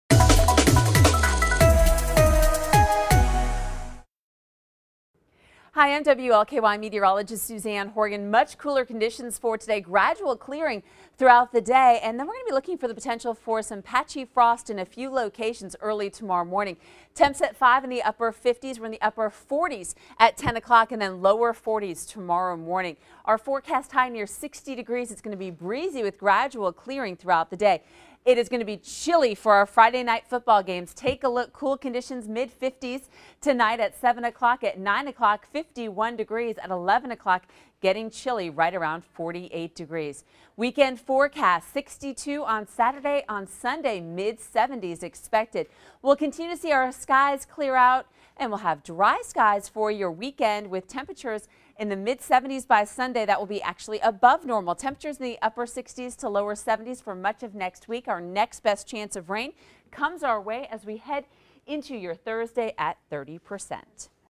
Cool for Friday Night Football
WLKY meteorologist Susanne Horgan has your forecast.